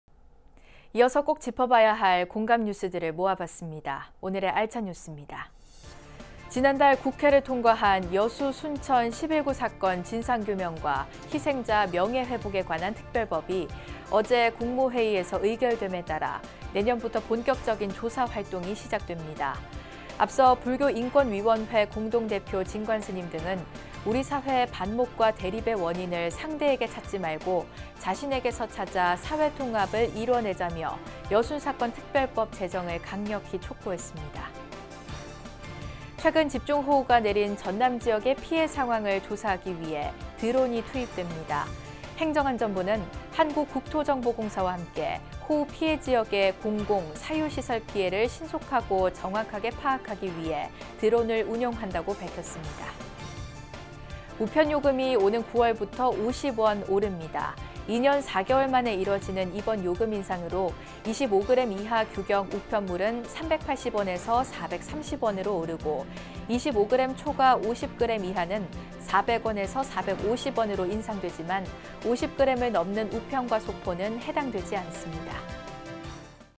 [BBS뉴스] 7월 14일(수) 알찬뉴스
1. 여순사건, 내년부터 본격 진상규명...불교인권위 촉구

지난달 국회를 통과한 '여수·순천 10·19사건 진상규명과 희생자 명예회복에 관한 특별법'이 어제(13일) 국무회의에서 의결됨에 따라 내년부터 본격적인 조사활동이 시작됩니다.

앞서 불교인권위원회 공동대표 진관스님 등은 우리 사회의 반목과 대립의 원인을 상대에게 찾지말고 자신에게서 찾아 사회통합을 이뤄내자며 '여순사건 특별법' 제정을 강력히 촉구했습니다.

2. 행안부, 호우 피해지역 드론 활용 피해조사 실시

최근 집중 호우가 내린 전남지역의 피해 상황을 조사하기 위해 드론이 투입됩니다.

행정안전부는 한국국토정보공사와 함께 호우 피해지역의 공공·사유 시설 피해를 신속하고 정확하게 파악하기 위해 드론을 운용한다고 밝혔습니다.

3. 과기부, 우편요금 9월부터 50원 인상

우편요금이 오는 9월부터 50원 오릅니다.

2년 4개월만에 이뤄지는 이번 요금 인상으로 25그램 이하 규격 우편물은 380원에서 430원으로 오르고, 25그램 초과 50그램 이하는 400원에서 450원으로 인상되지만 50그램을 넘는 우편과 소포는 해당되지 않습니다.